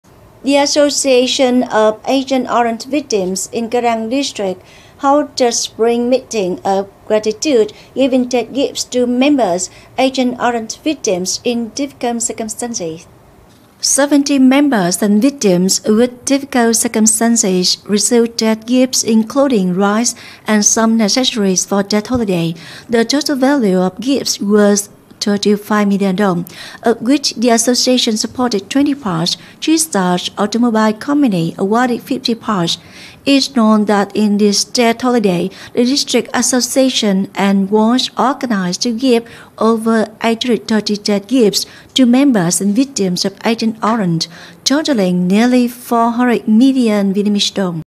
Cái Răng district held a spring meeting of gratitude | Cần Thơ News
The Association of Agent Orange Victims in Cái Răng District held a spring meeting of gratitude, giving Tết gifts to members, Agent Orange victims in difficult circumstances.